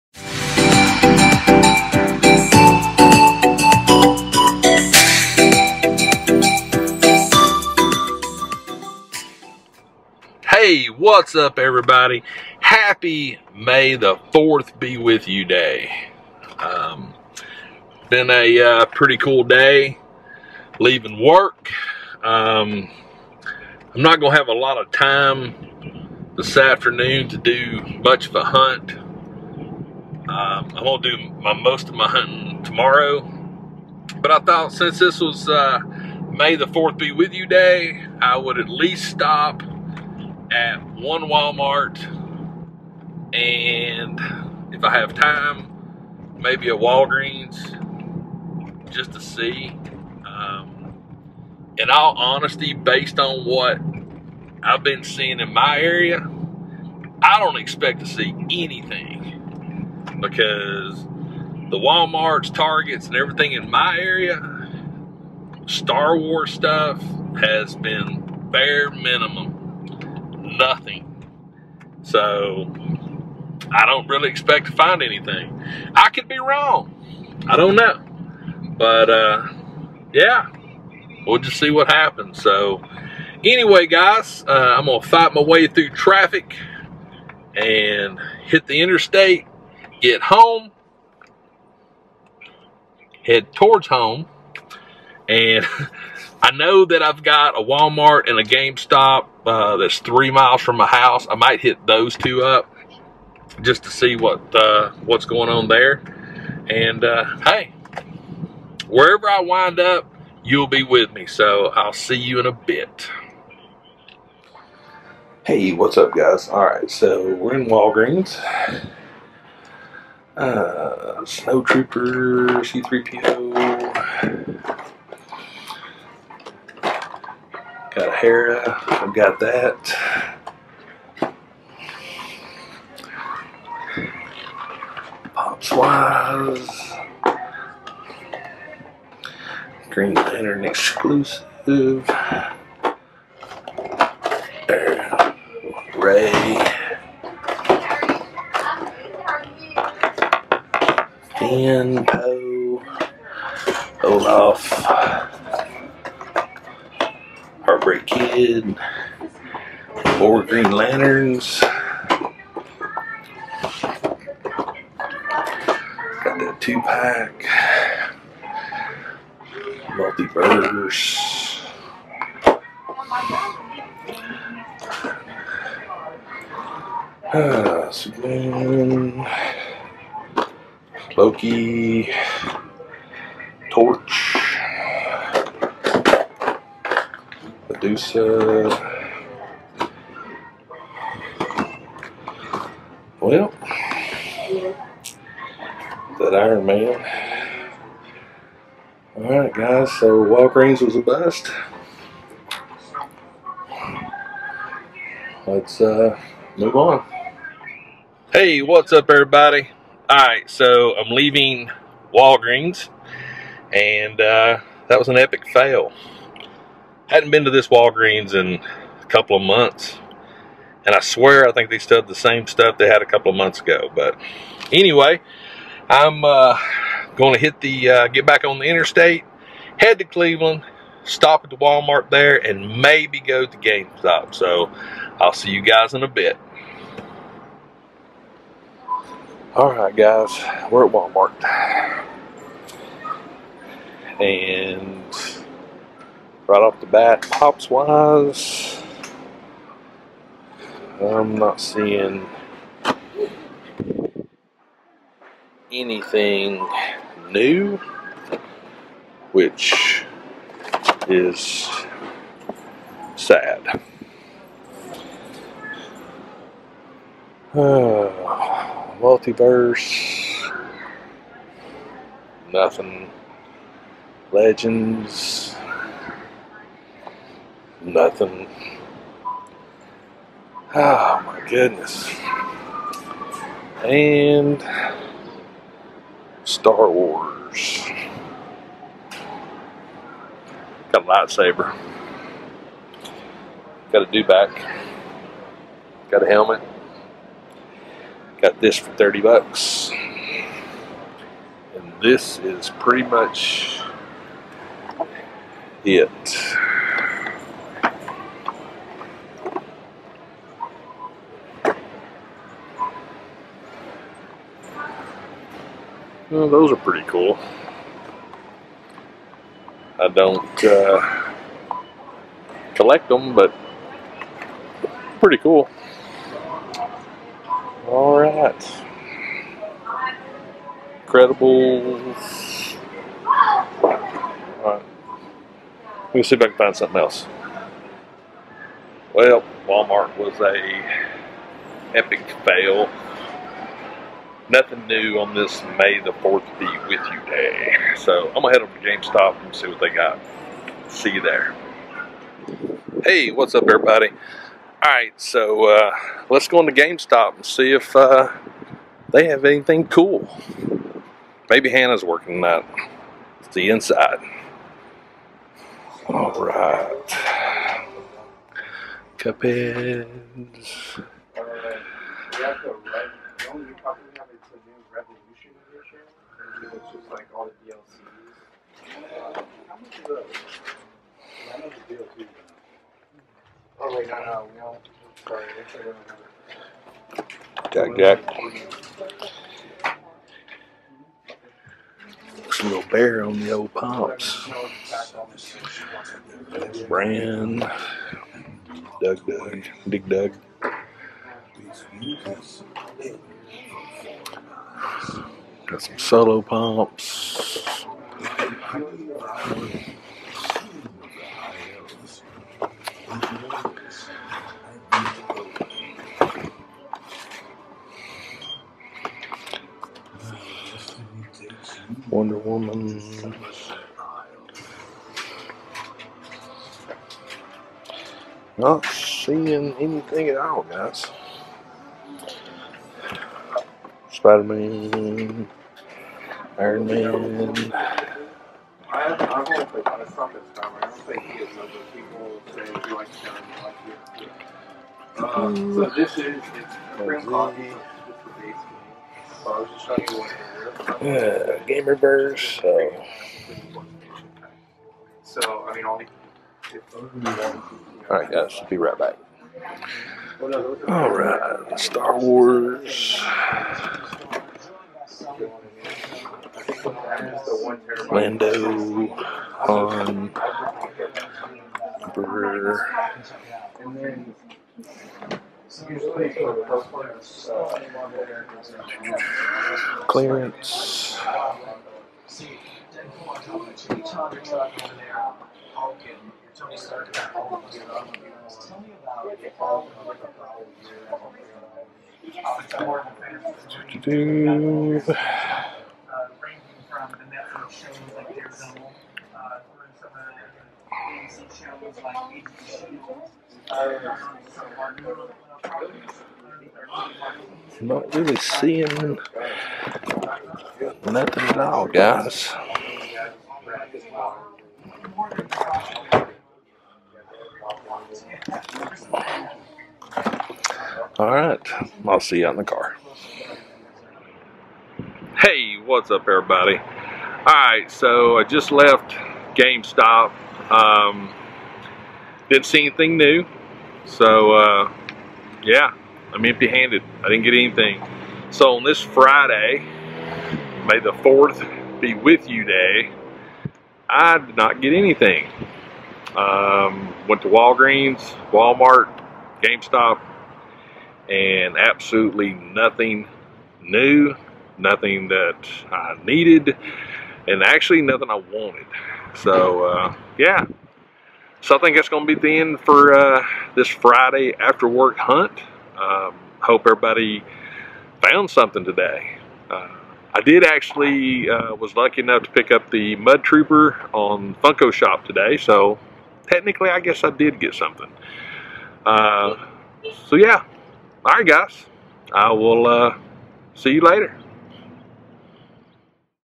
Episode 106 - May The Forth Be With You After Work Hunt for Black Series and Funko Pops
Well today is Star Wars day, so I thought I would try and do a small after work hunt at Walgreens, Walmart and GameStop.

Send Mail To:
Greg Cook